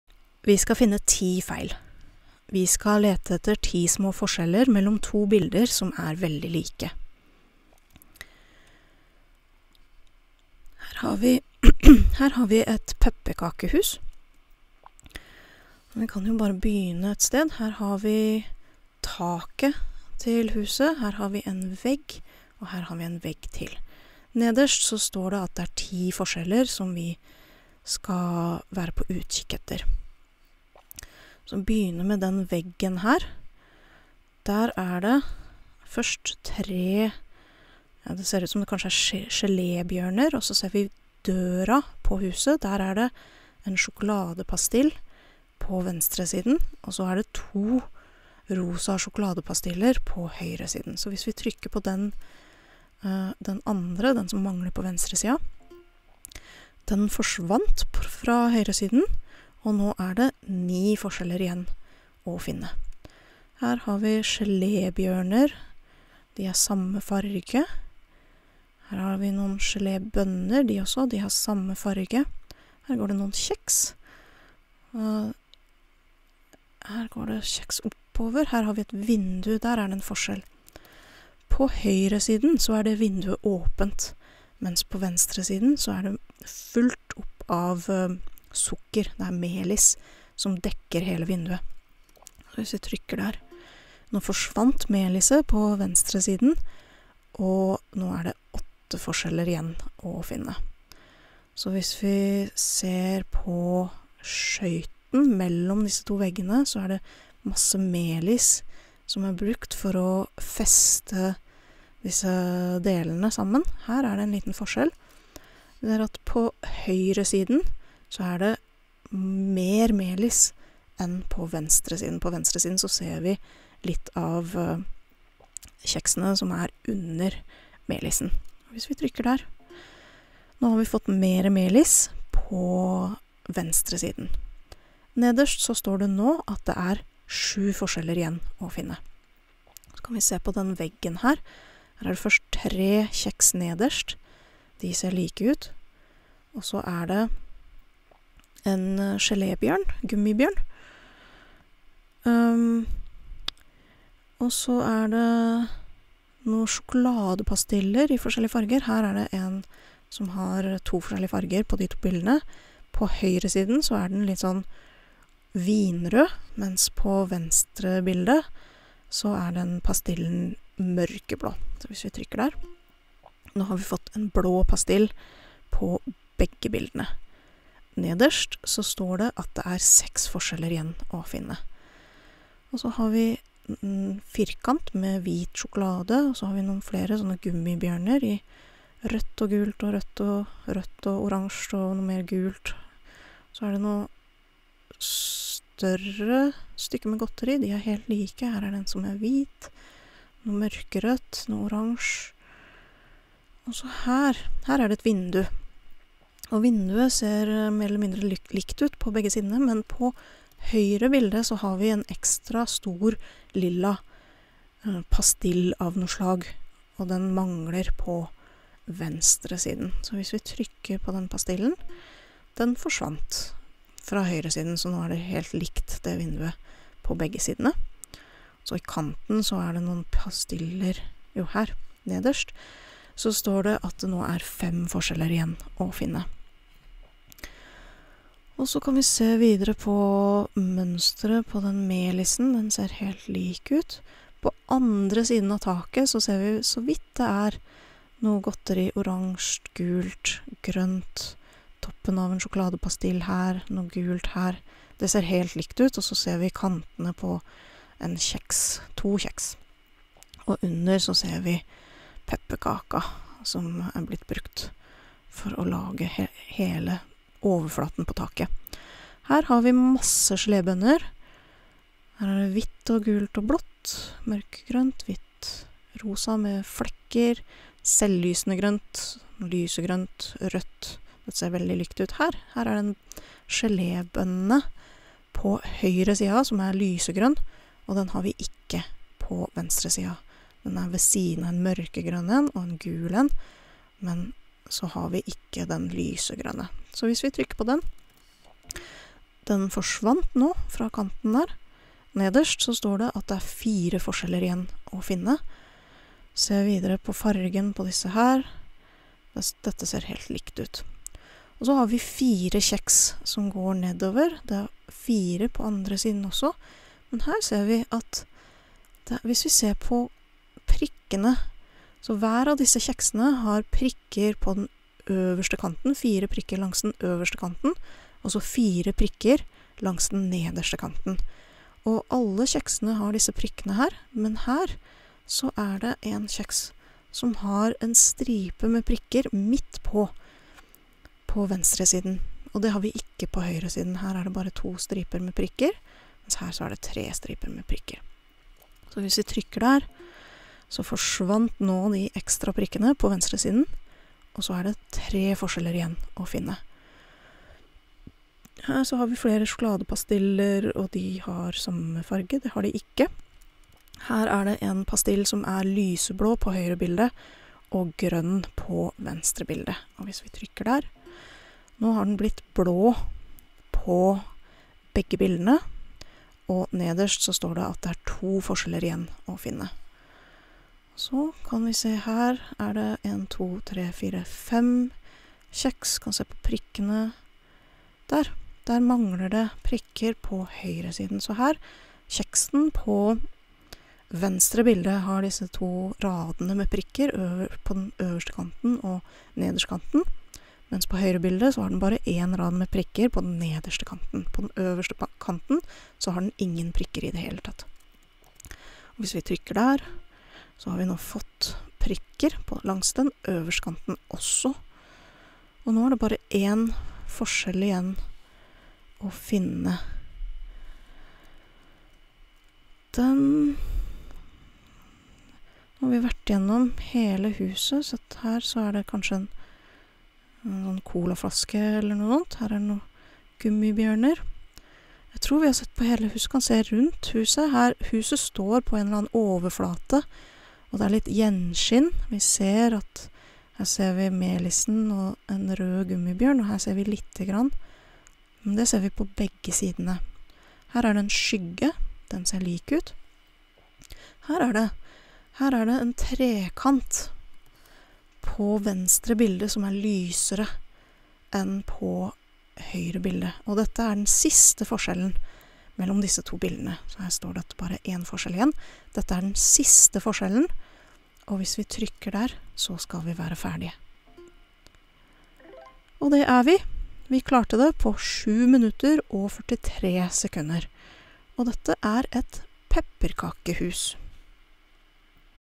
#33: Et pepperkakehus (Spot the Differences)
A native Norwegian speaker looks for differences between two similar pictures, providing descriptions in simple, clear Norwegian.

The pictures come from Smithsonian Magazine's "Spot the Difference Daily" game. In today's video we're playing the game published on December 25, 2021, where we're looking for differences between two pictures of a gingerbread house.

The game was discontinued on January 1st, 2023.

This video is probably lower-intermediate level.

As with all the videos on this channel, the goal is to provide listening practice for people to learn Norwegian through comprehensible input.